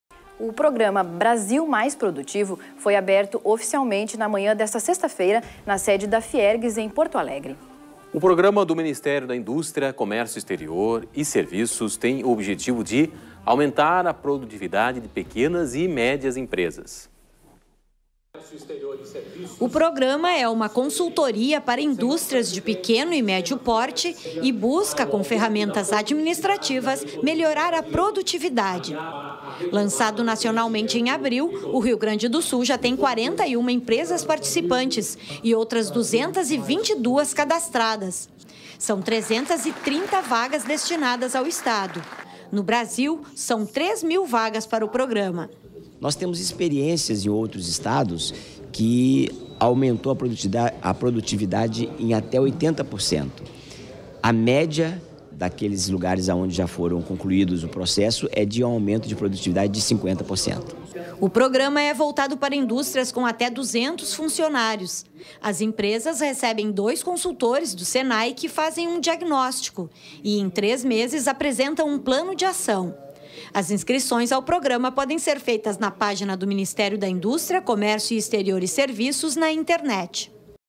Lançado programa que pretende aumentar produtividade de micro e pequenas empresas
O Programa Brasil Mais Produtivo foi aberto oficialmente na manhã desta sexta-feira na sede da Fiergs, em Porto Alegre. O programa do Ministério da Indústria, Comércio Exterior e Serviços tem o objetivo de aumentar a produtividade de pequenas e médias empresas.